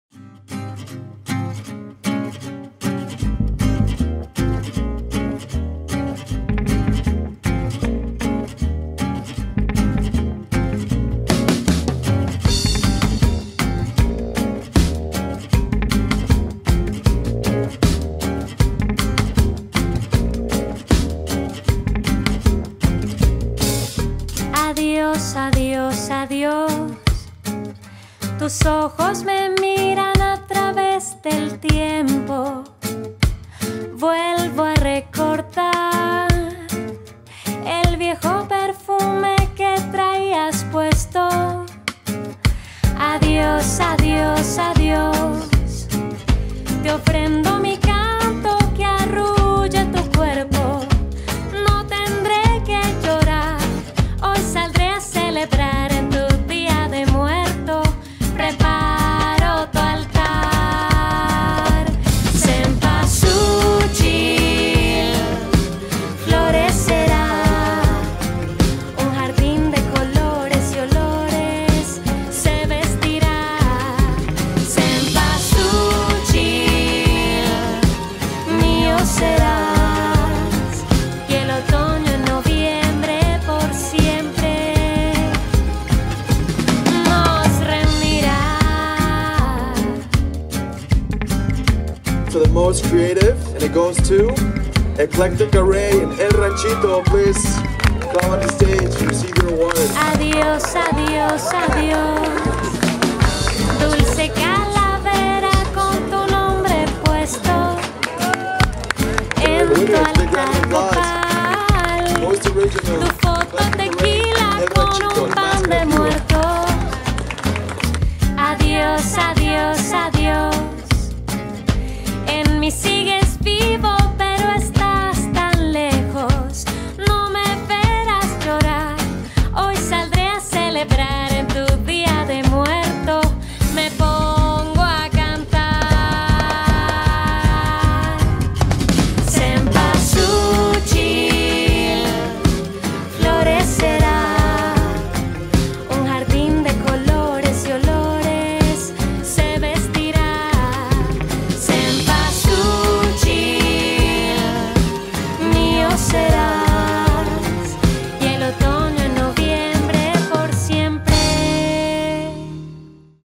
Day of the Dead Altar Competition at Flora Farms
We won most creative at the annual día de los muertos competition, it  was a beautiful altar honoring the flora farms dogs that have passed away. We were given $10,000 pesos to donate to El Ranchito, a local pet rescue. Enjoy this short video to see this joyful and emotional day.

Or shop on Etsy